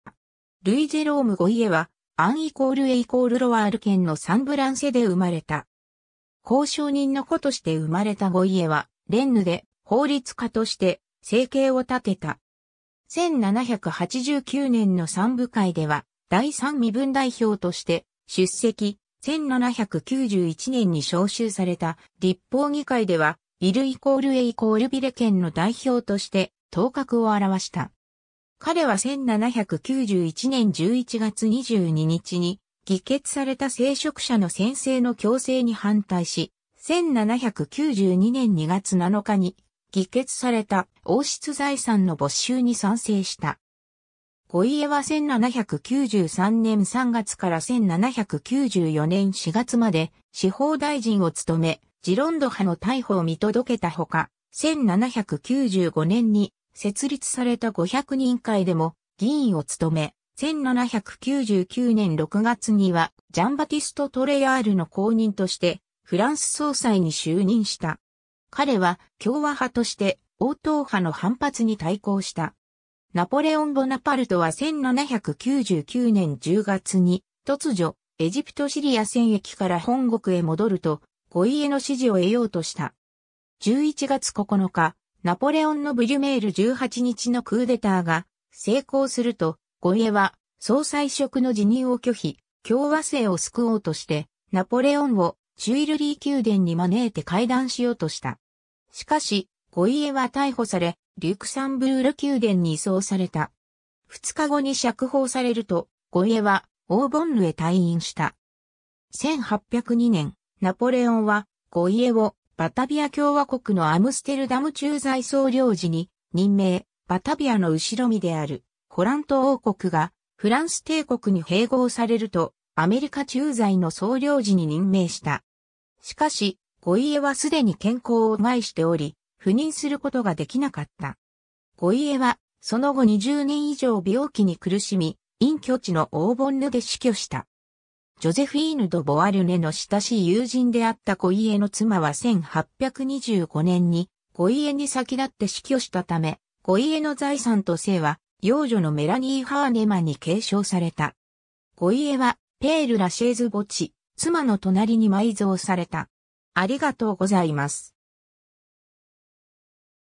ルイ＝ジェローム・ゴイエ
アンドル＝エ＝ロワール県出身の人物
ルイ＝ジェローム・ゴイエ（フランス語: Louis-Jérôme Gohier、1746年2月27日、1830年5月29日）は、フランス革命時期の政治家。
 ルイ＝ジェローム・ゴイエはアンドル＝エ＝ロワール県のサンブランセで生まれた。
公証人の子として生まれたゴイエはレンヌで法律家として生計を立てた。
1789年の三部会では第三身分代表として出席、1791年に召集された立法議会ではイル＝エ＝ヴィレーヌ県の代表として頭角を現した。
彼は1791年11月22日に議決された聖職者の宣誓の強制に反対し、1792年2月7日に議決された王室財産の没収に賛成した。
 ゴイエは1793年3月から1794年4月まで司法大臣を務め、ジロンド派の逮捕を見届けたほか、1795年に設立された五百人会でも議員を務め、1799年6月にはジャン＝バティスト・トレヤールの後任としてフランス総裁に就任した。
彼は共和派として王党派の反発に対抗した。
 ナポレオン・ボナパルトは1799年10月に突如エジプト・シリア戦役から本国へ戻ると、ゴイエの支持を得ようとした。
11月9日、ナポレオンのブリュメール18日のクーデターが成功すると、ゴイエは総裁職の辞任を拒否、共和政を救おうとしてナポレオンをテュイルリー宮殿に招いて会談しようとした。
しかし、ゴイエは逮捕され、リュクサンブール宮殿に移送された。
2日後に釈放されると、ゴイエはオーボンヌへ退隠した。
 1802年、ナポレオンはゴイエをバタヴィア共和国のアムステルダム駐在総領事に任命、バタヴィアの後身であるホラント王国がフランス帝国に併合されるとアメリカ駐在の総領事に任命した。
しかし、ゴイエはすでに健康を害しており、赴任することができなかった。
 ゴイエはその後20年以上病気に苦しみ、隠居地のオーボンヌで死去した。
ジョゼフィーヌ・ド・ボアルネの親しい友人であったゴイエの妻は1825年にゴイエに先立って死去したため、ゴイエの財産と姓は養女のメラニー・ハーネマンに継承された。
 ゴイエはペール・ラシェーズ墓地、妻の隣に埋蔵された。